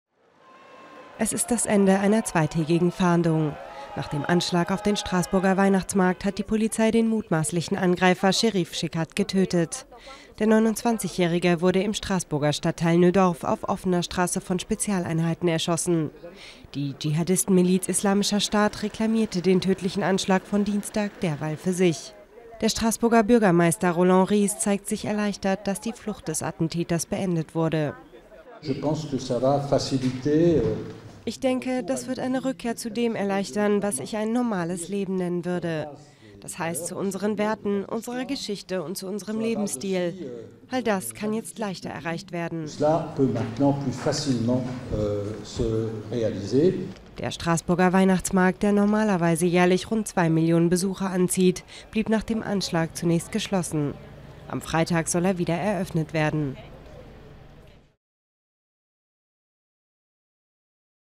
Polizei tötet Straßburg-Attentäter Chekatt
Zwei Tage nach dem Anschlag in Straßburg ist der mutmaßliche Angreifer Chérif Chekatt getötet worden. Der 29-Jährige wurde am Donnerstagabend im Viertel Neudorf südöstlich des Straßburger Zentrums von Polizisten erschossen.